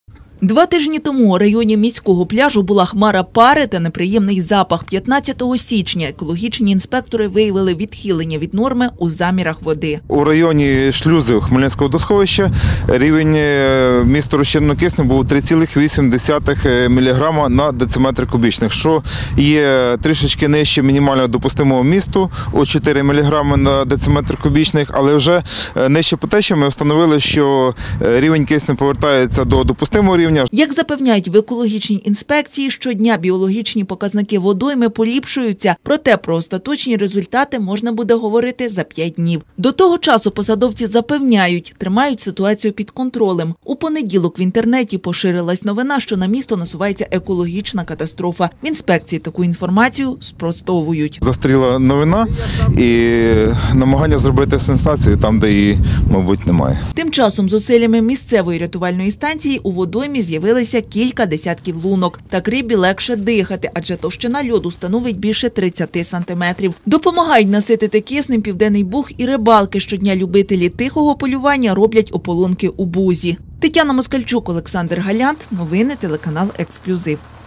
Екологи досліджували воду у Південному Бузі
Клуби пару та різкий запах. Так два тижні тому виглядала картина на Південному Бузі. Тоді містяни викликали екологів на річку, аби ті з’ясували, чи є небезпека для риби. Лабораторні дослідження показали занижений рівень кисню у воді.